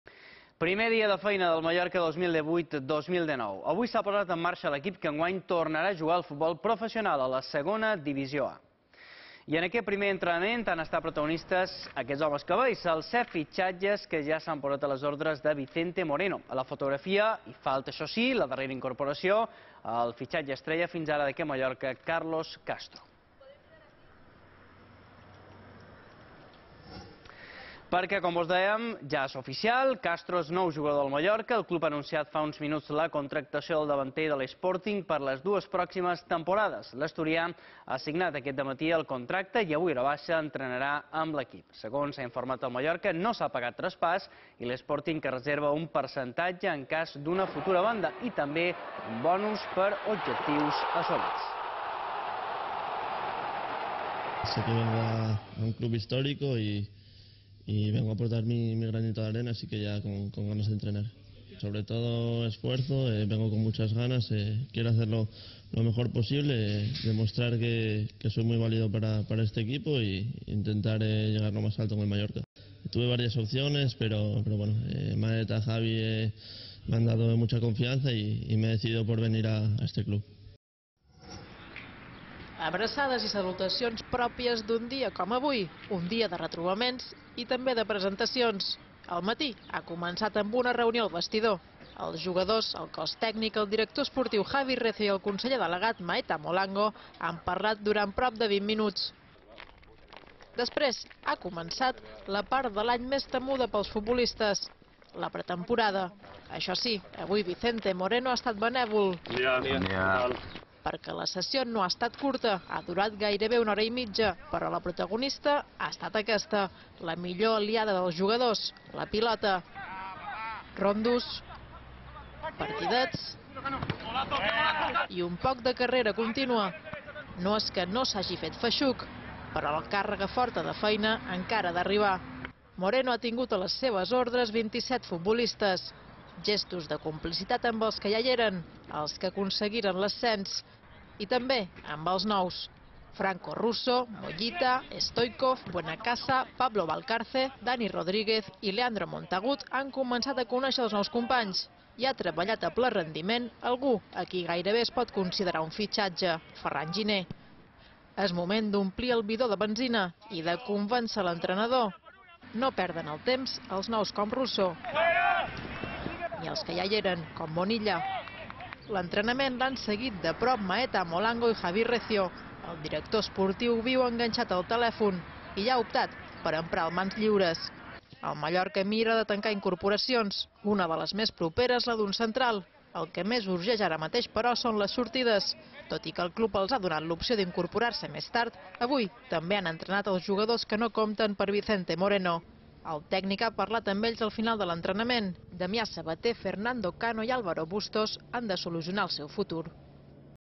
Carlos Castro ja és jugador del Reial Mallorca
El club fa oficial la contractació del davanter procedent de l’Sporting de Gijón. L’equip ha iniciat aquest dimarts la pretemporada